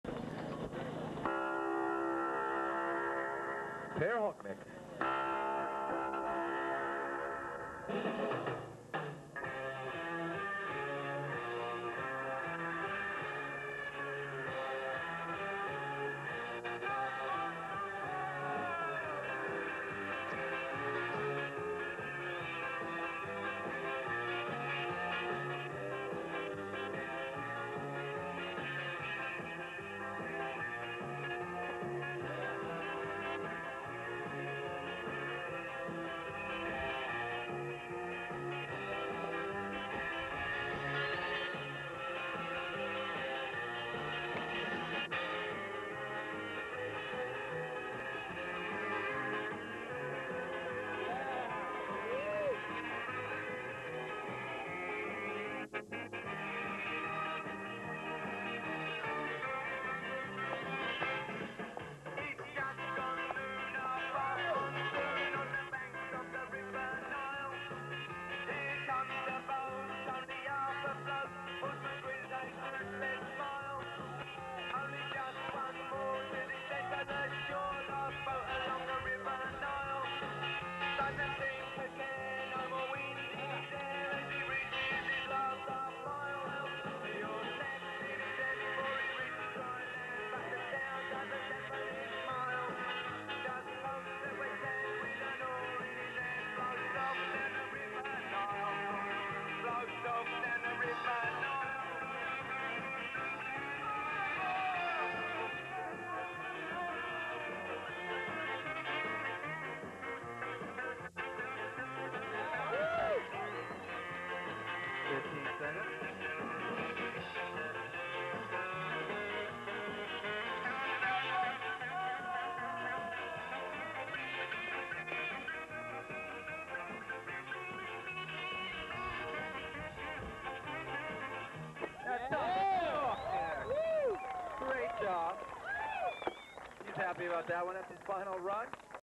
Per Holknekt - run 3, 1984
Per Holknekt skates his third run at a contest in 1984 in San Francisco.